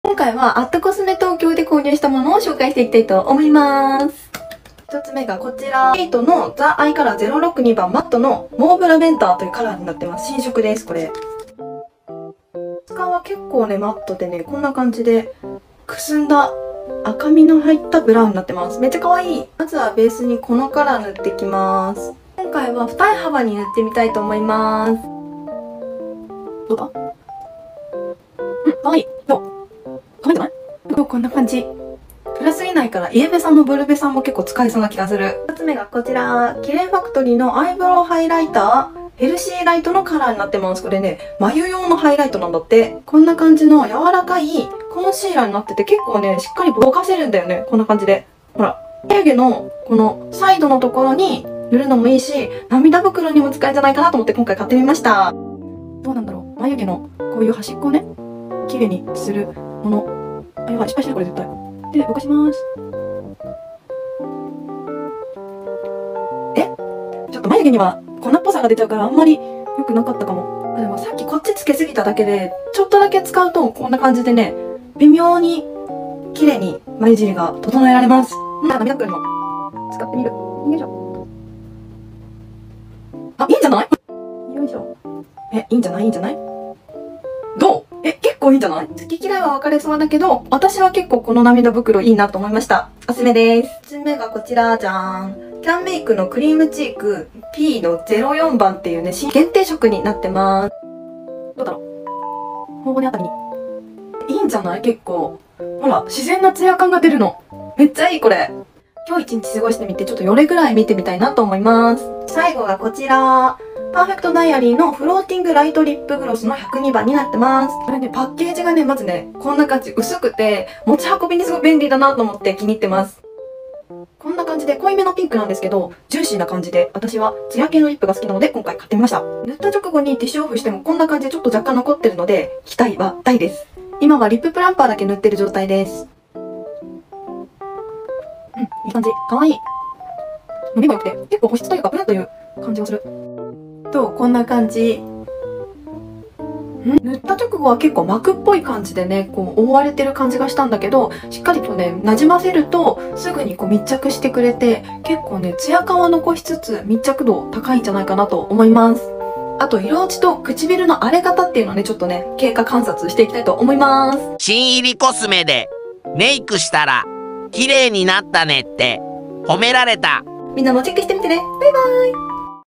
【購入品紹介】＠cosmeで買ったコスメでメイクしたら綺麗になったねって言われた
こんにちは！ひまりです🌻
今回は＠cosme購入品をご紹介しました！